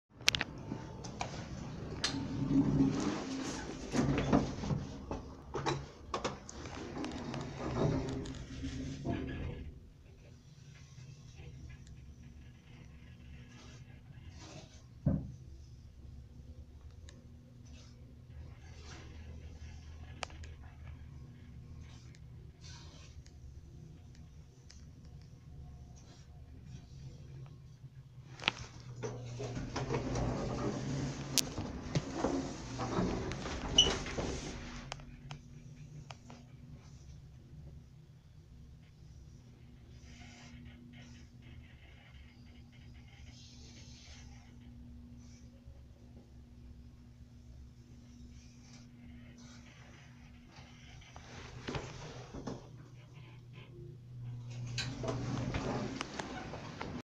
三菱エレベーター
高知市付近某ビルにあります。まだ未更新のままで運転しています。機種は中期エレペットアドバンスvです。
メーカー:三菱
設置年:1980年代後半
定員:6名
積載:450kg
キャンセル機能:なし
アナウンス:なし
戸閉ブザー:なし
到着ベル音:なし
ボタン確認音:なし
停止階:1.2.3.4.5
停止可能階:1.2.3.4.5
管理会社:エレックスサービス
撮影:NEC LAVIETabE PC-TE508BA (2021年9月19日撮影)